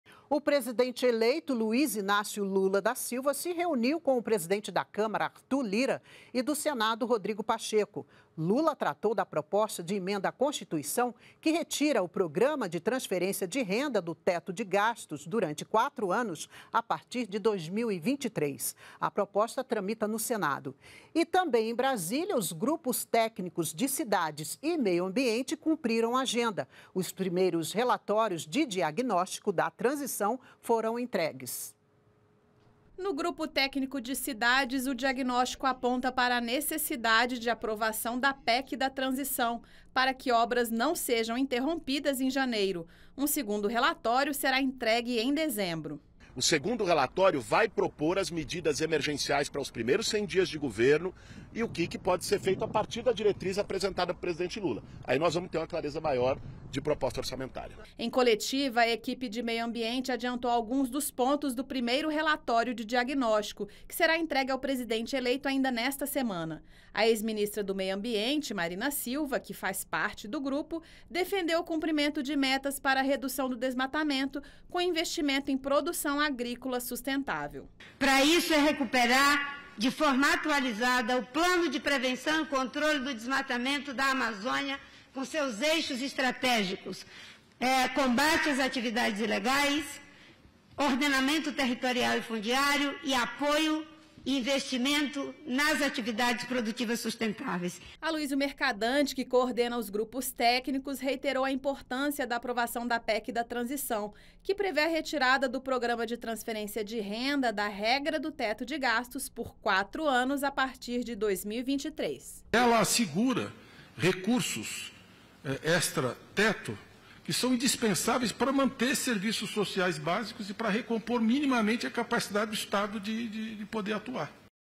Política | PEC da Transição
1º.12.2022 - O presidente eleito, Luiz Inácio Lula da Silva, se reuniu com o presidente da Câmara, Arthur Lira, e do Senado, Rodrigo Pacheco. Lula tratou da Proposta de Emenda à Constituição que retira o programa de transferência de renda do teto de gastos durante 4 anos, a partir de 2023. A PEC tramita no Senado.